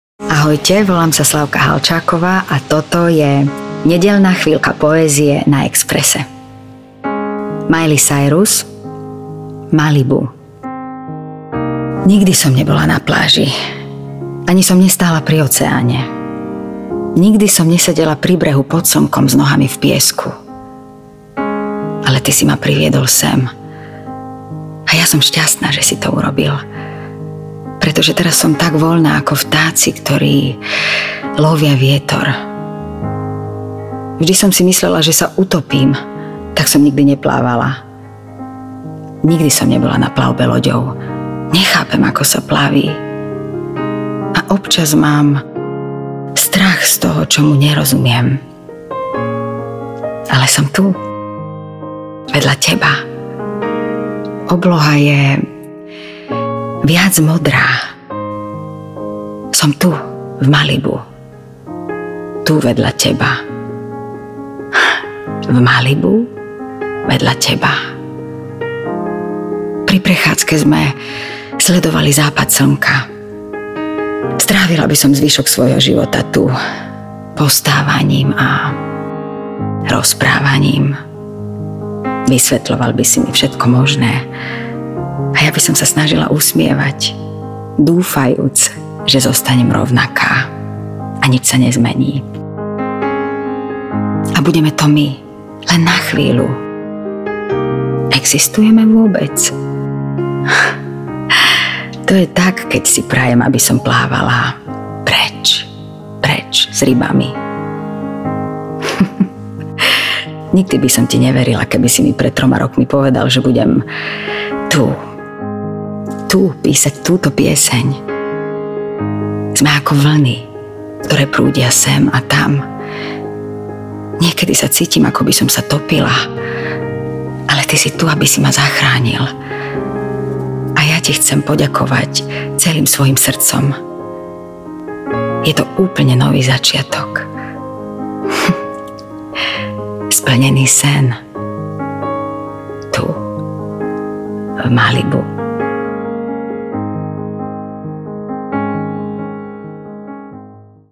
Éterická Slávka Halčáková vás vezme do Malibu...
Vypočujte si najnovší diel našej Nedeľnej chvíľky poézie!